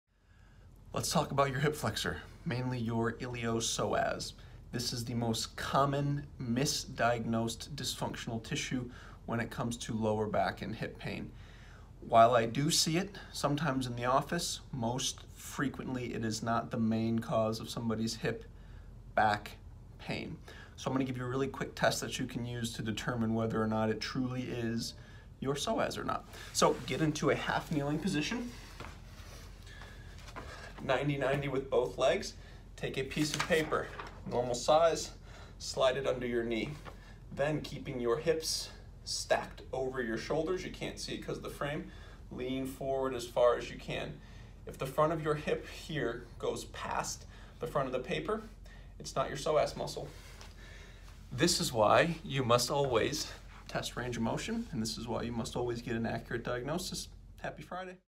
Hip Flexor Flexibility Test- Is your hip flexor causing you pain?
Here is a simple test to determine whether or not your hip flexor, mainly your iliopsoas, is lacking flexibility. It’s important to know which tissue is causing your pain. Without an accurate diagnosis, it’s impossible to get out of pain.